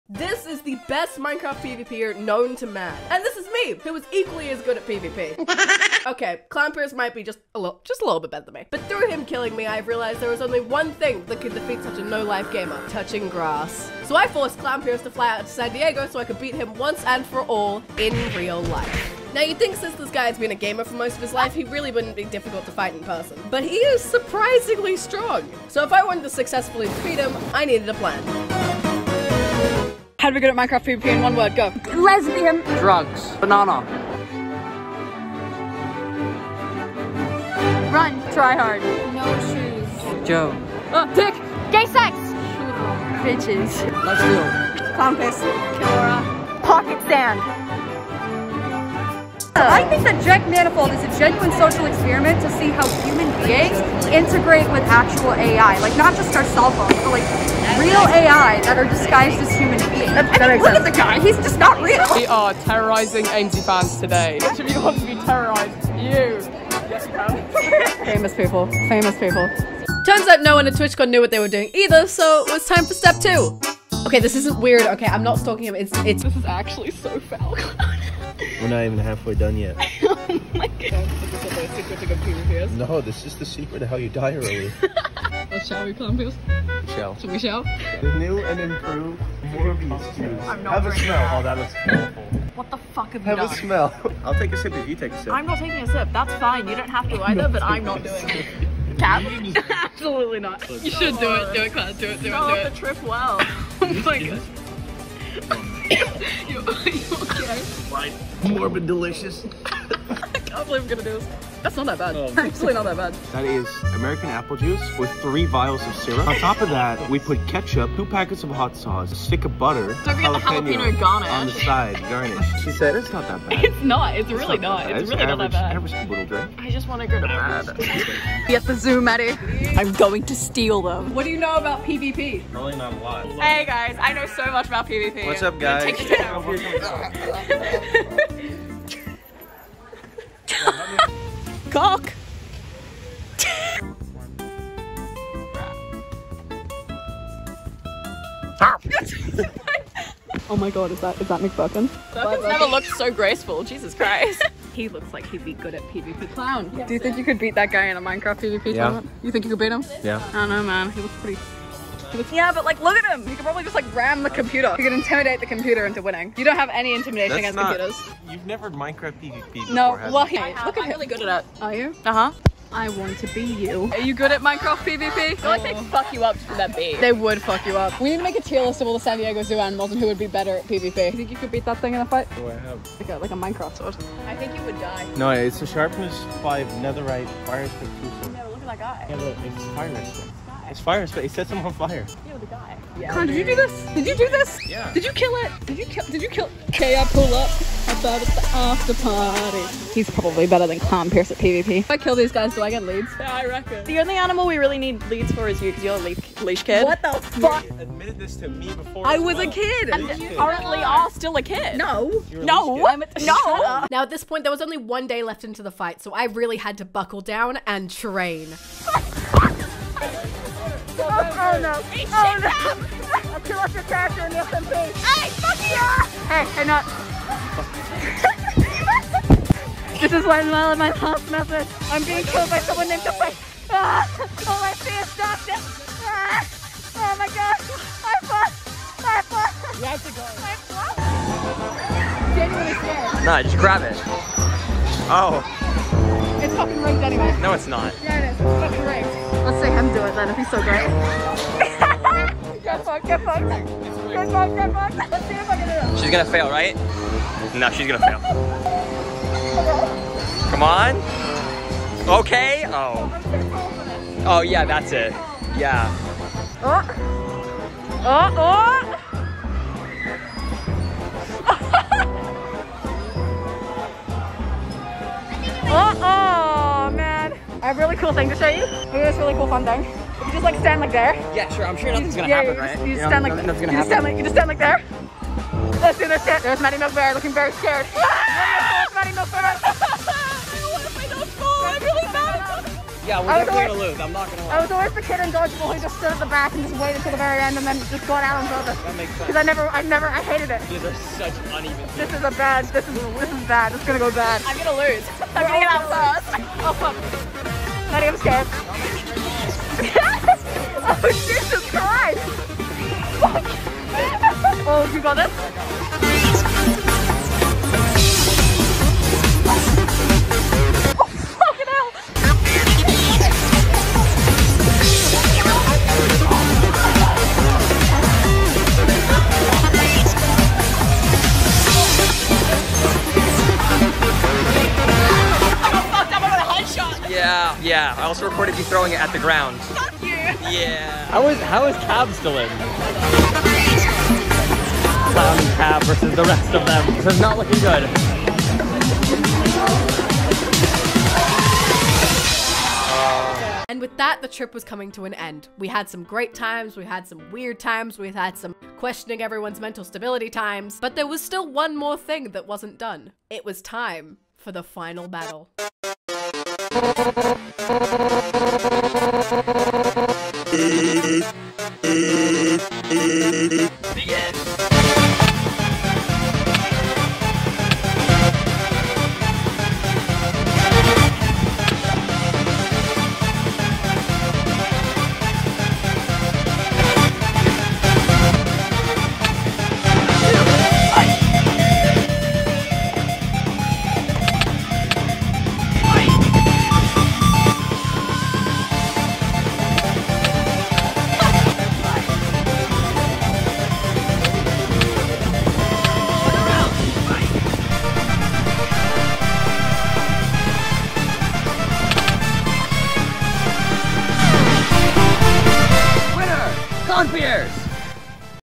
Fighting Minecraft's Deadliest PvPer In Real Life
Fighting Minecraft's Deadliest PvPer In Real Life at TwitchCon! @ClownPierce

Hope you all enjoyed this quick video from TwitchCon and America :)!!

I would love to see you over on my twitch!  ^

Minecraft SMP & Twitch Con content inspired by Dream and TommyInnit from the DreamSMP.

Edited by me and dqwnfx